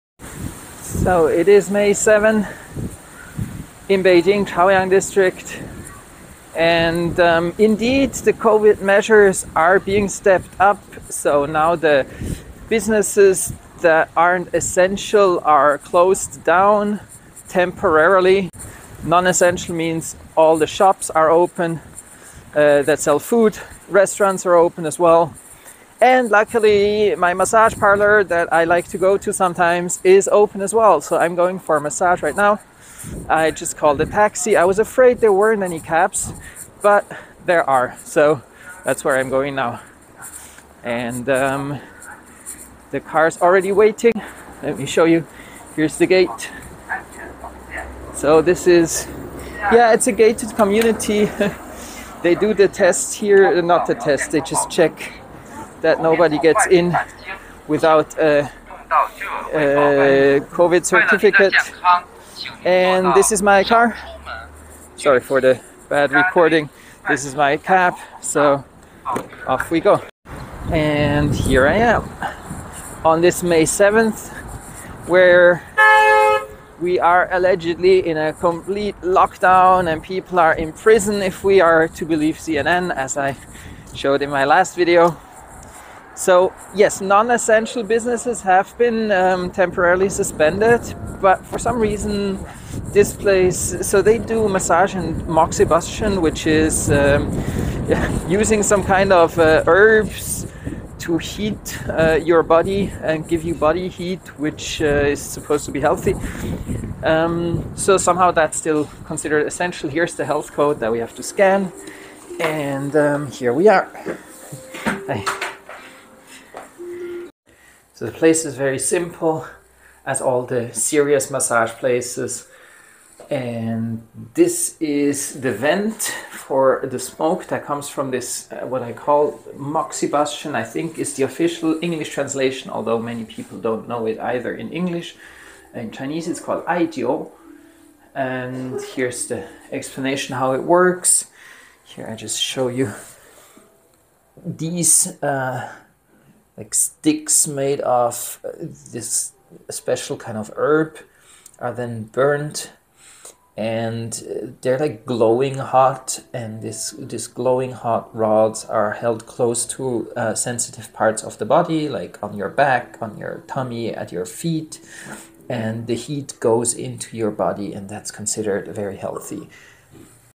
Beijing, China still no Lockdown: going to moxibustion and massage on May 7
As some media seem to paint a bleak picture of Beijing, and China in general, I send another quick update. Yes, non-essential shops and cinemas are closed, but not just food markets and restaurants remain open, even my wellness place continues to operate.